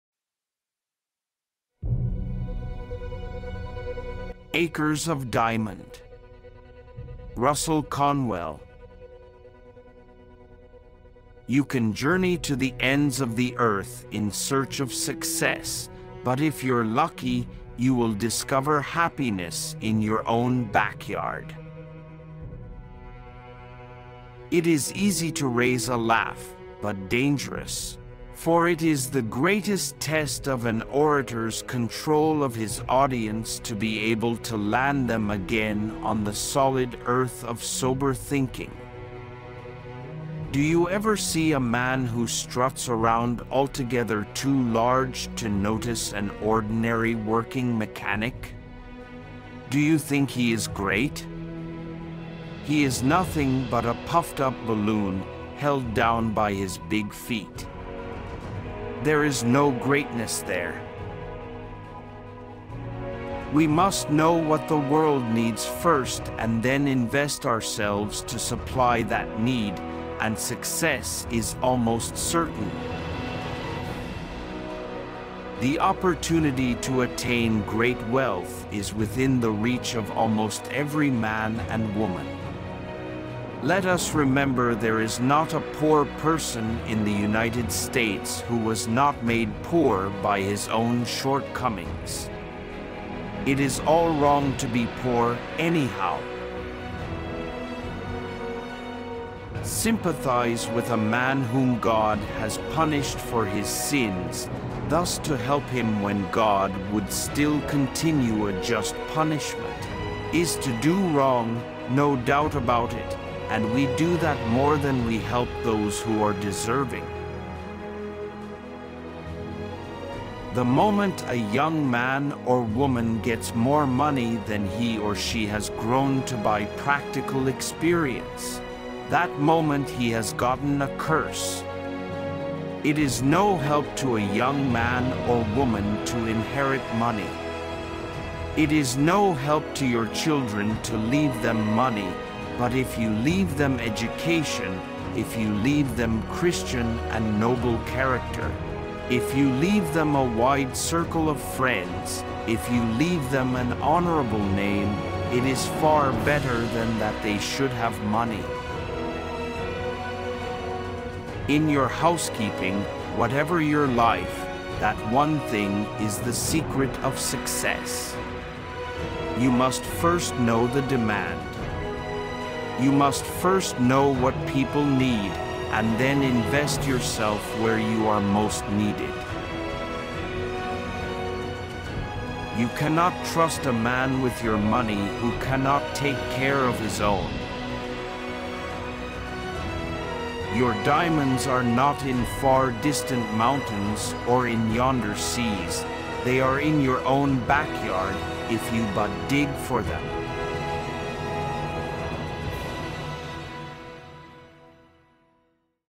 Quotes by Russell H Conwell, Acres Of Diamond
Russell Herman Conwell was an American Baptist minister, orator, philanthropist, author, lawyer, and writer. He is best remembered as the founder and first president of Temple University in Philadelphia, as the Pastor of The Baptist Temple, and for his inspirational lecture, "Acres of Diamonds".

In Memories Of Russell H. Conwell, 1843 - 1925